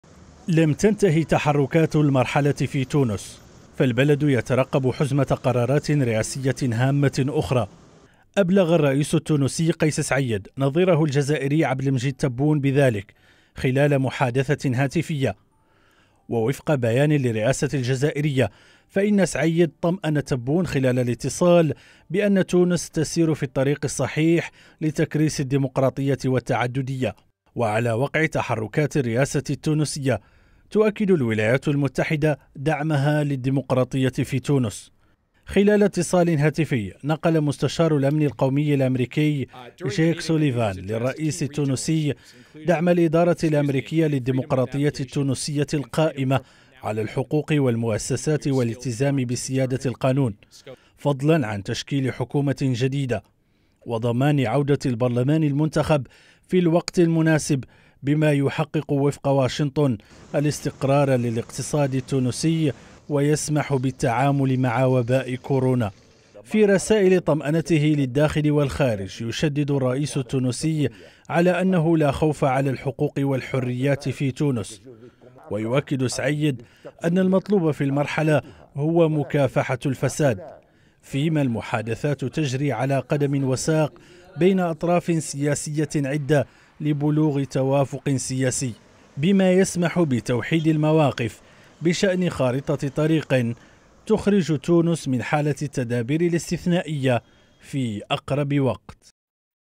ترقب في تونس لقرارات رئاسية هامة قريبا
بحث الرئيس التونسي قيس سعيد مع نظيره الجزائري عبد المجيد تبون، آخرَ تطورات الأوضاع في تونس. وقالت الرئاسة الجزائرية، إن سعيد أبلغ تبون بأن قرارات هامة ستصدر قريبا. من جهة أخرى أكدت واشنطن دعمها للديمقراطية التونسية.